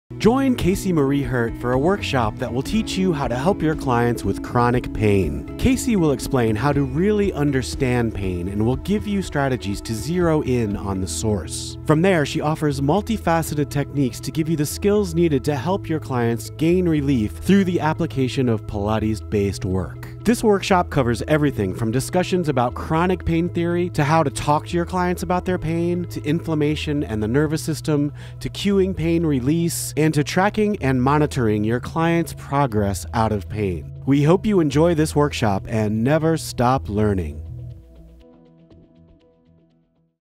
Pain Body: The Essentials by Casey Marie Herdt
In this online Pilates workshop, you will explore the ways we as Pilates teachers can be a guide and aide to our clients who experience chronic pain and dis-ease. By taking a multifaceted approach to understanding pain, and creative ways of reframing it, we can help our clients along the path of the "healer's journey". By getting to the root of the issue we can lift the veils that cloud clarity, and systematically apply wonderful tools from the Pilates studio to help them shed the skin of pain, layer by layer.